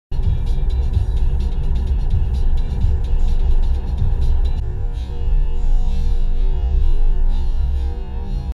close encounter with a owl
Driving home later at night and then this dare devil owl was all like "come at me bro!"